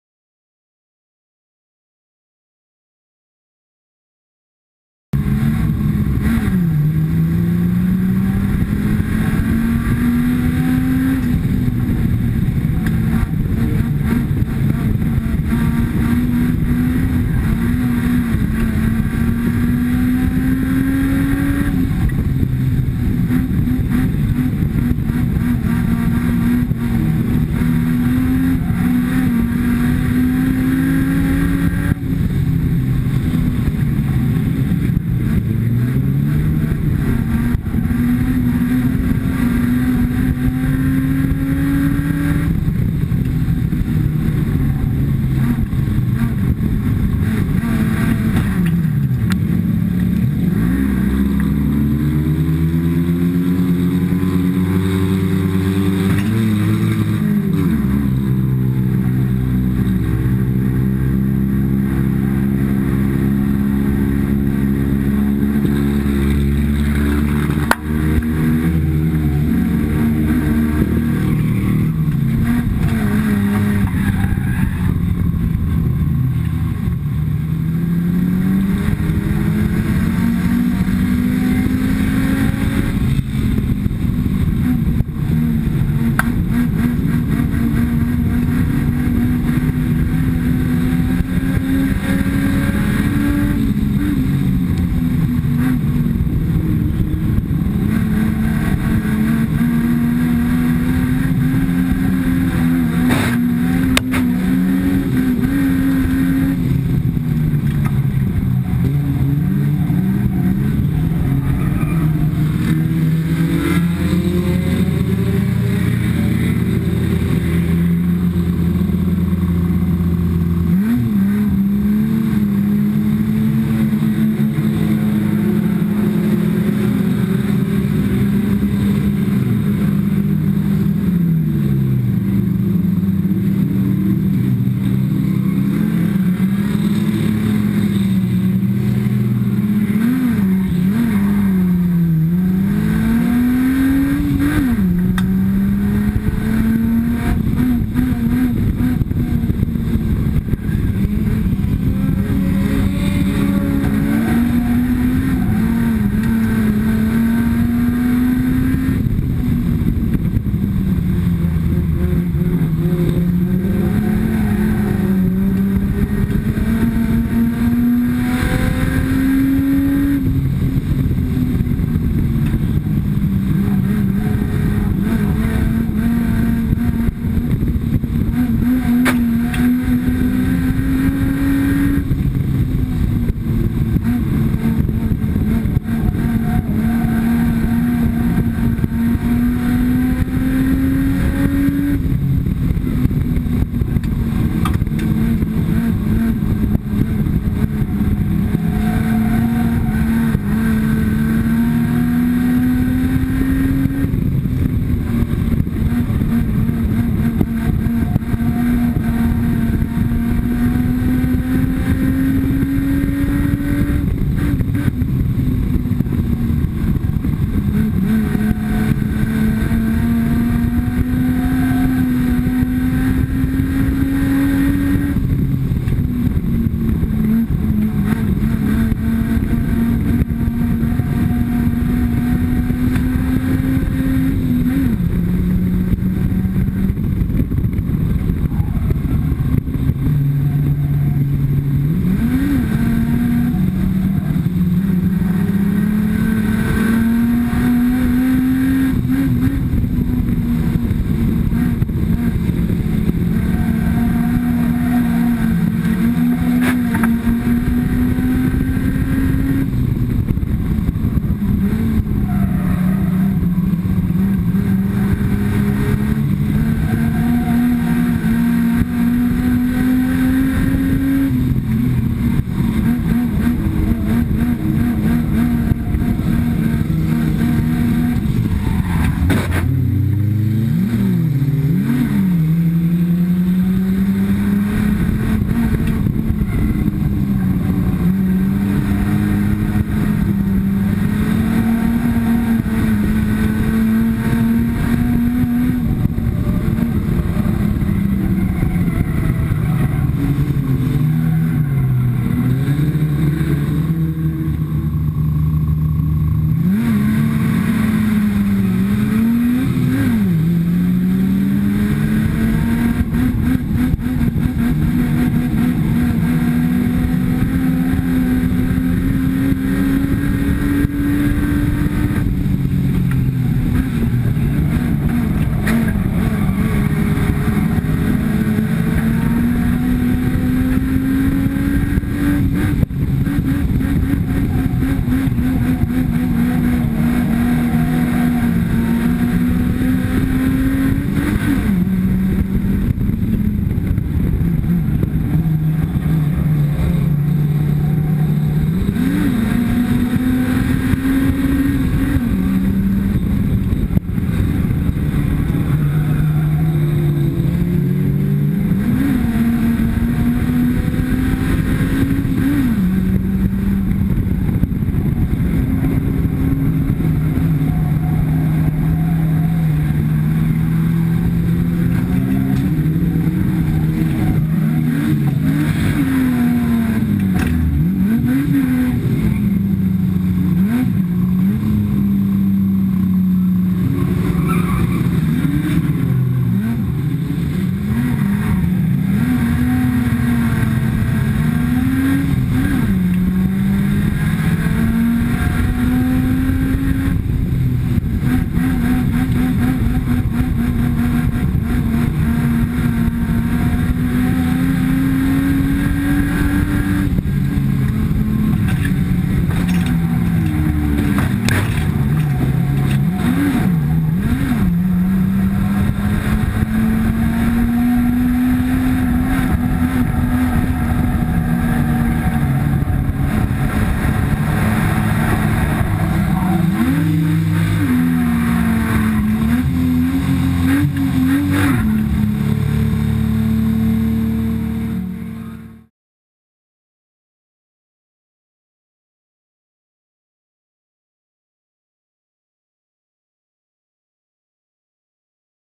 Onboard #211 Dennis Vorkink - World Cup Unlimited Banger Racing Warneton 21-6-2015
Onboard by: #211 Dennis Vorkink - World Cup Warneton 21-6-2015